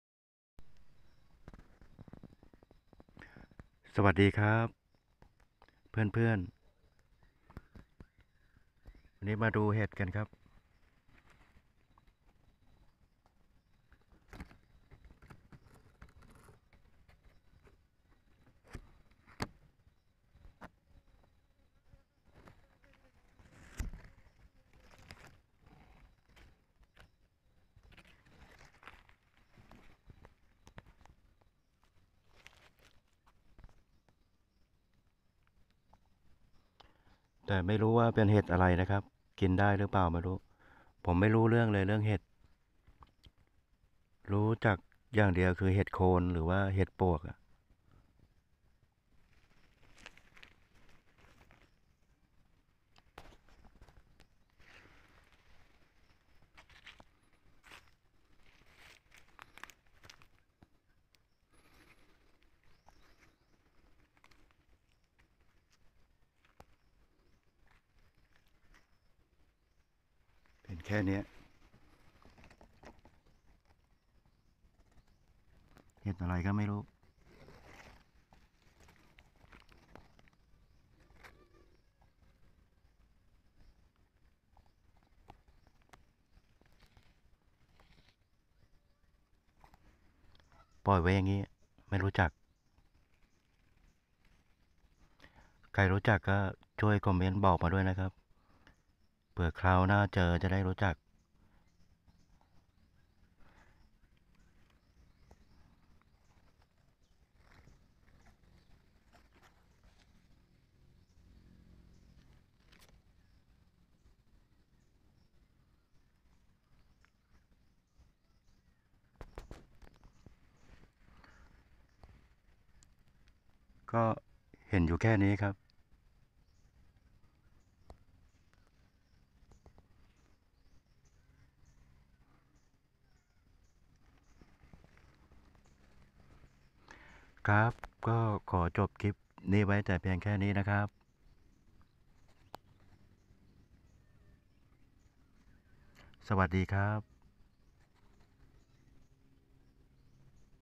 เห็ดก้นครก.🍄🍄🍄 👍😁😁😁(รายละเอียดเพิ่มเติมใต้คลิป)
ได้ชื่อมาแล้วครับ ชื่อเห็ดก้นครก ลักษณะตรงกลางดอกจะบุ๋มมองดูคล้ายก้นครก และยังเห็นเต่ากำลังกินเห็ดนี้อยู่ เต่ากินได้คนก็ต้องกินได้ แต่ก็ยังไม่กล้ากิน จนกว่าจะมีคนกินให้ดู ทางท้องถิ่นอื่นก็อาจเรียกแตกต่างกันไปครับ🤓?